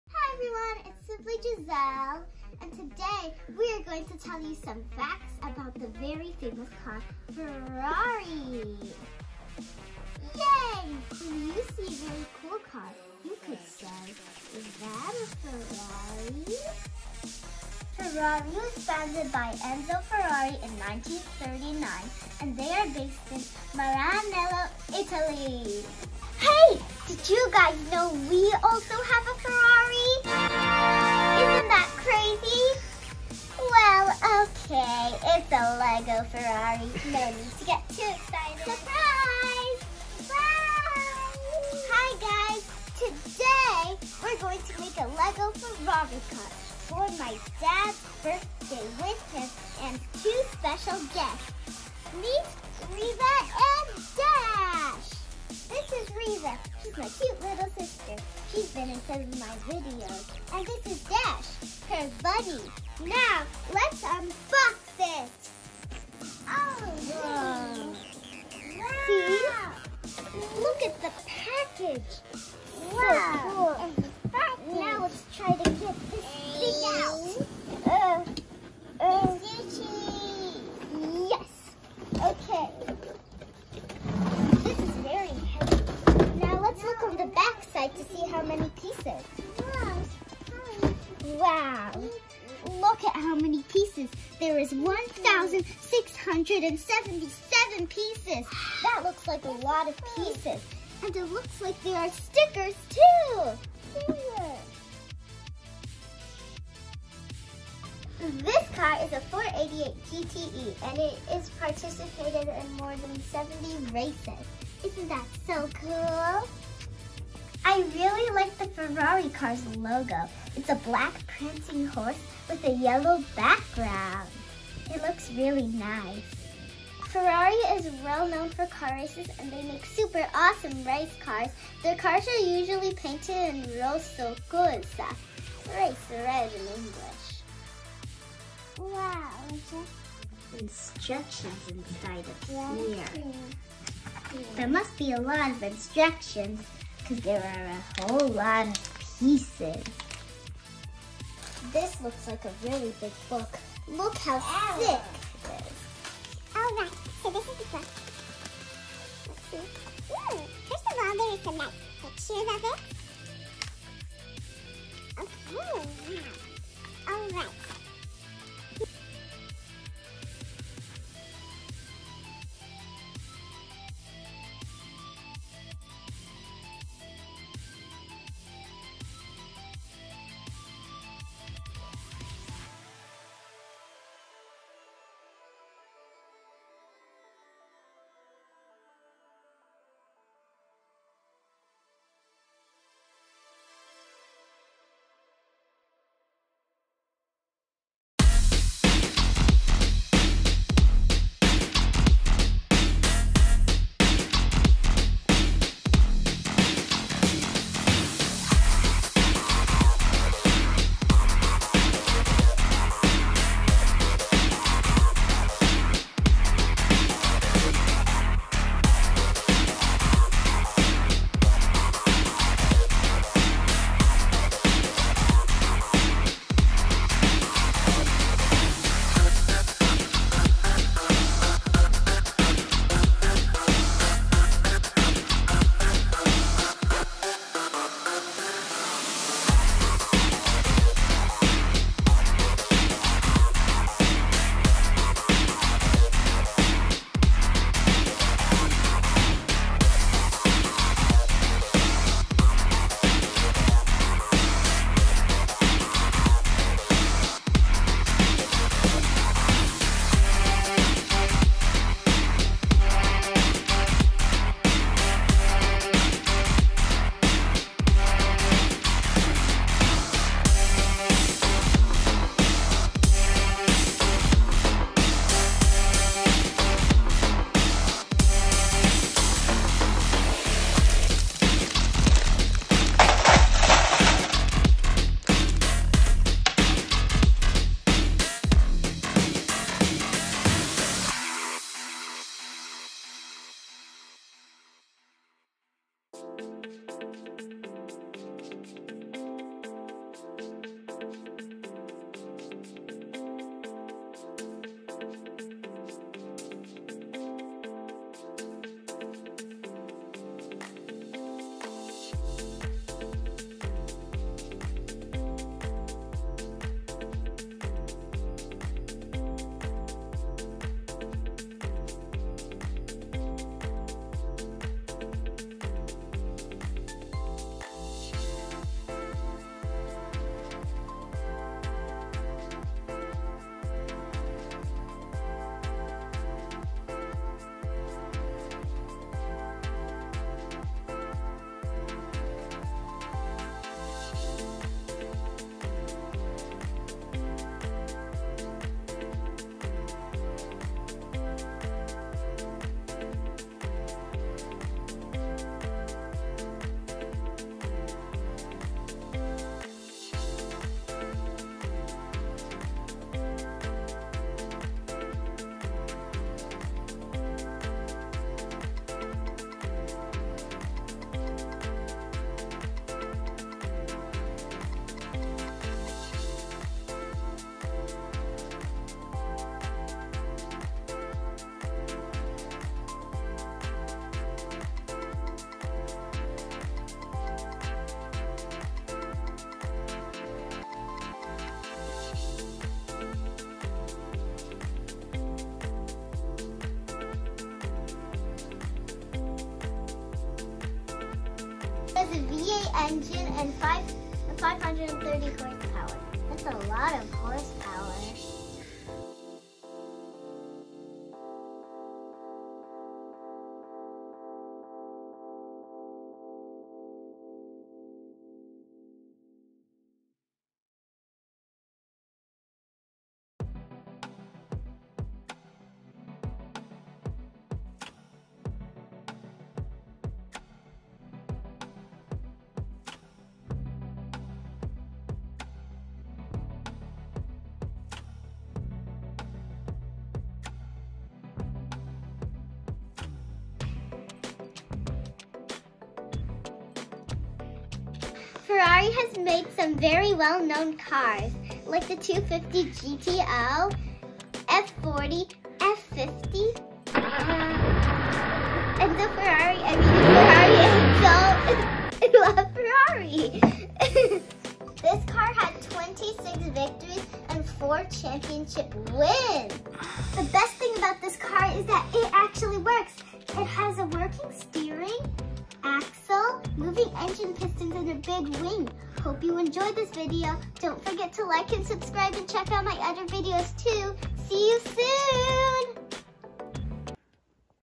Building a New Ferrari !
We build a Lego Technic Ferrari 488 GTE for my Dad’s Birthday . This has 1677 pieces and took more than 10 hours to build. I think it looks amazing for all the hard work. Do you like it?